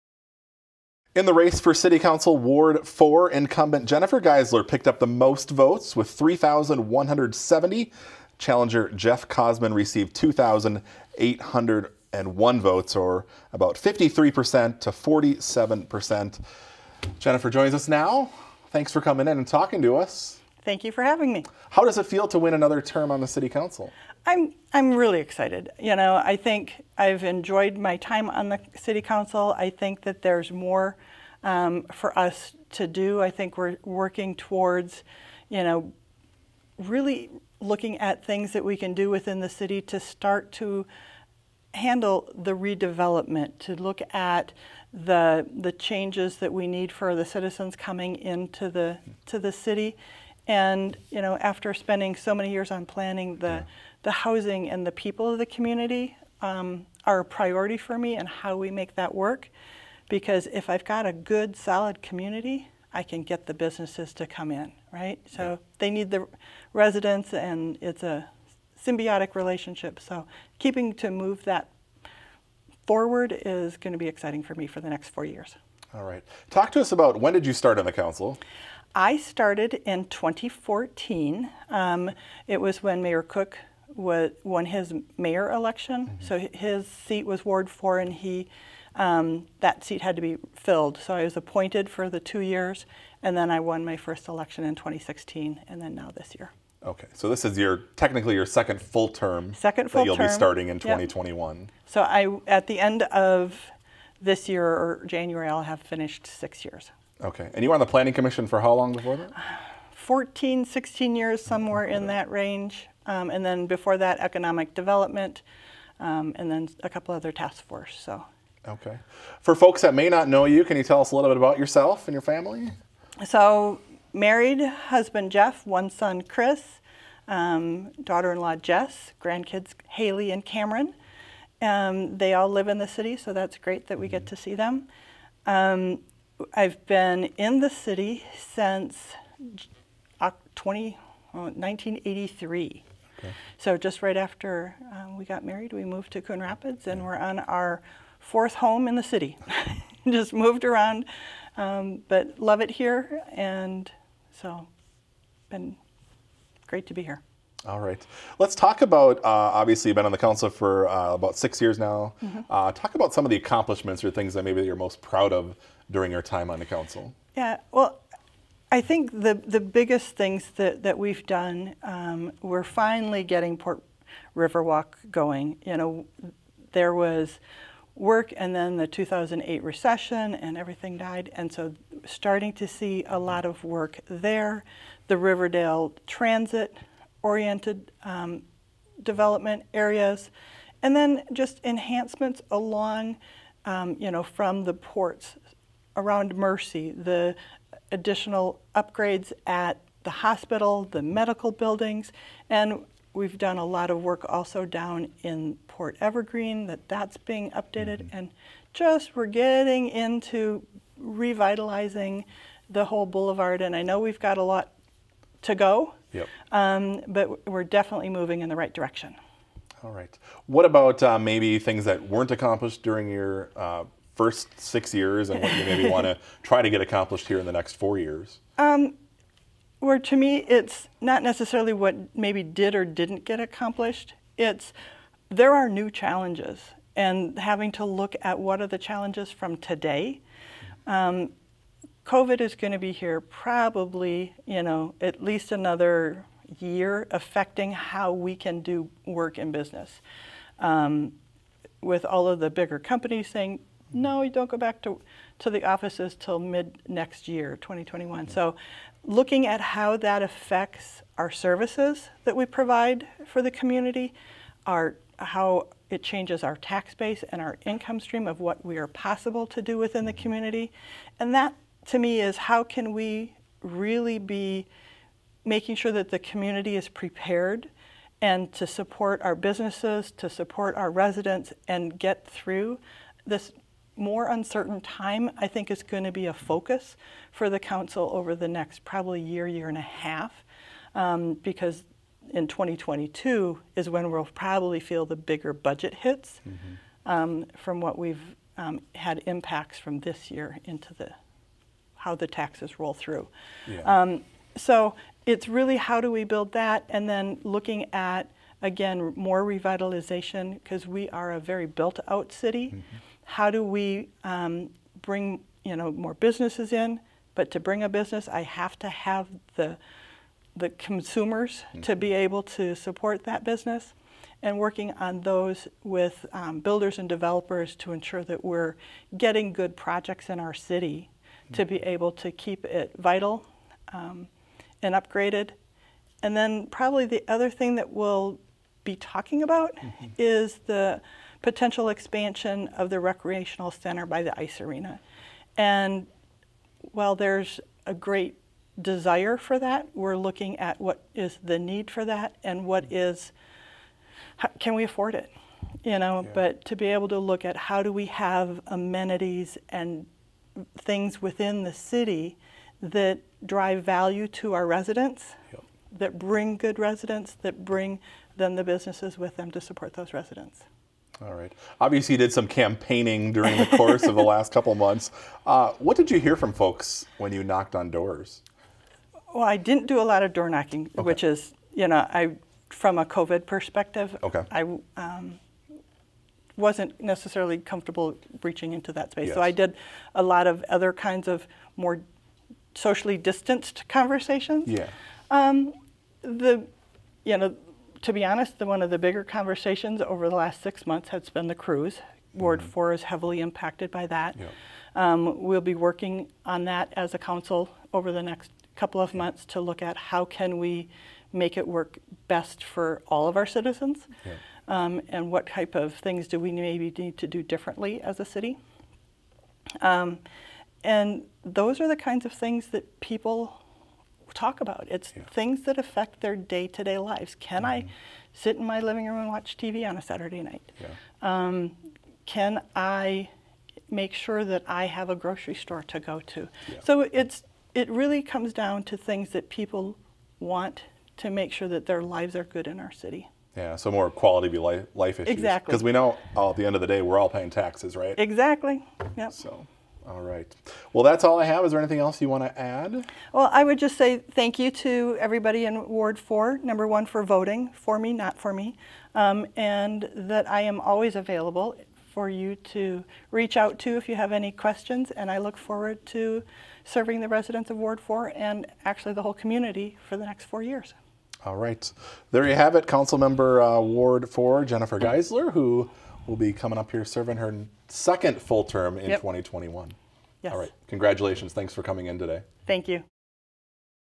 Jennifer Geisler - Ward 4 Winner
Meet Jennifer Geisler, the unofficial winner of the Coon Rapids City Council Ward Four race.